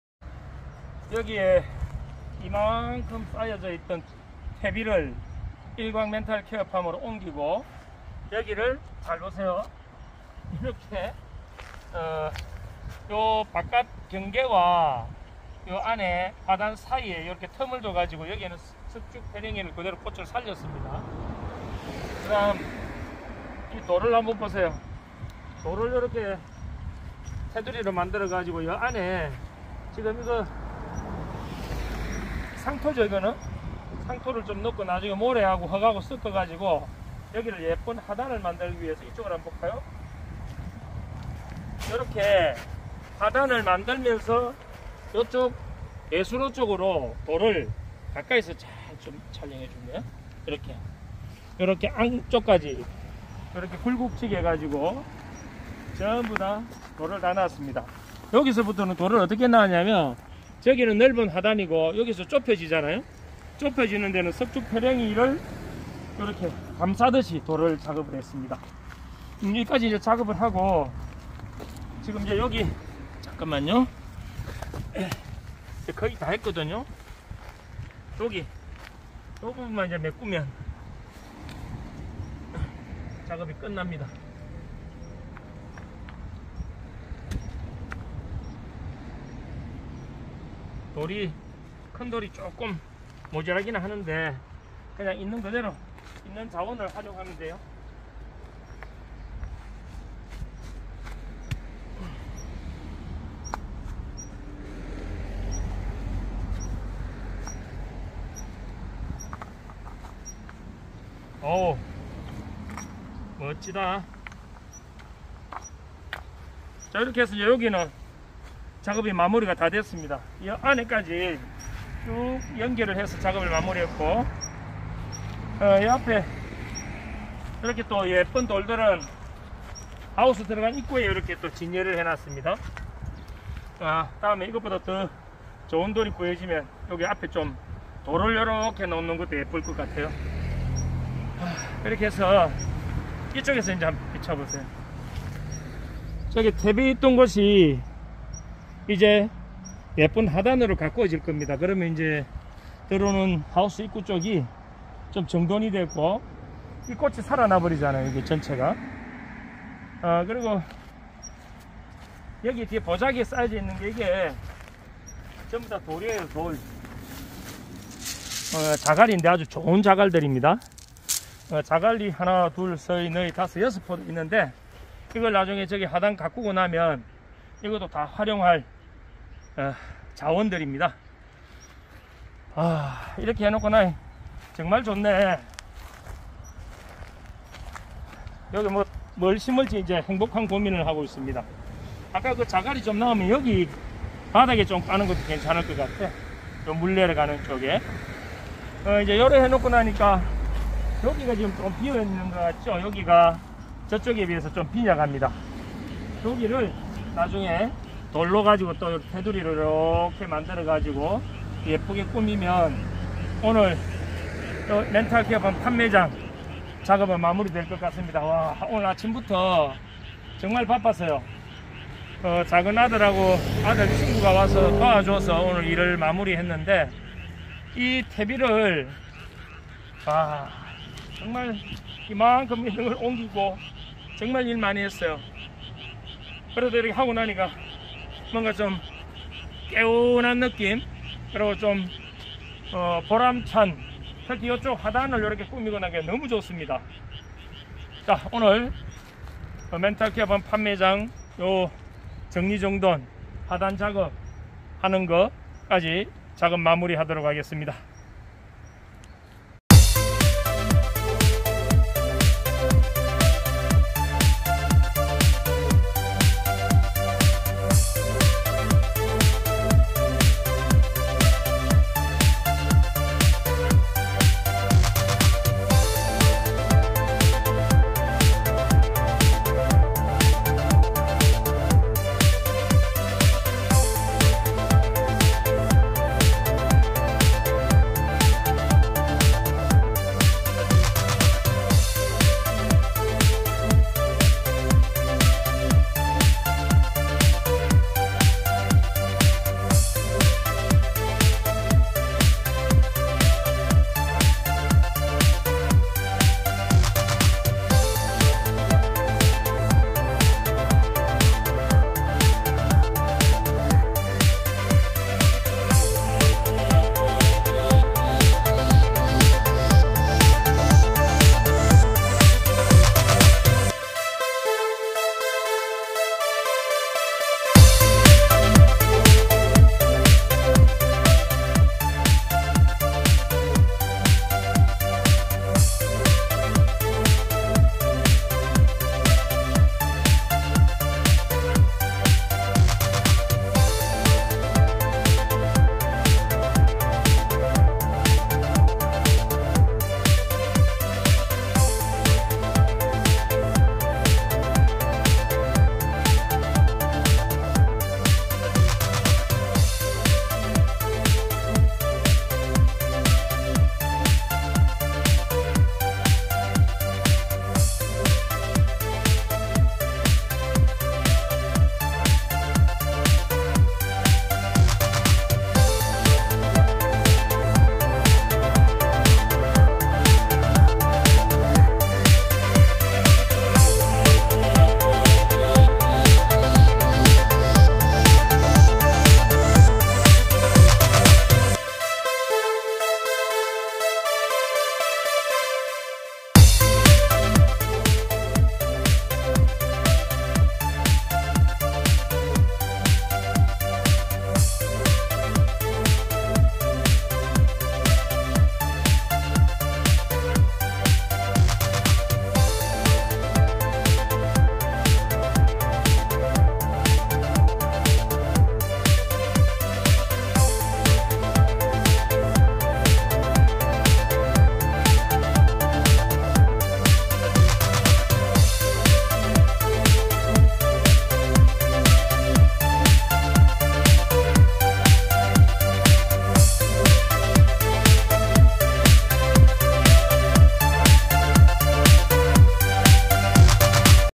무일푼으로 예쁜 화단 만들기/철마 땅/철마 농장 ※010.3838.1305
철마 멘탈케어팜 주차장 입구에 쌓아둔 퇴비를 일광 멘탈케어팜으로 옮기고 그 자리를 예쁜 화단으로 꾸몄습니다.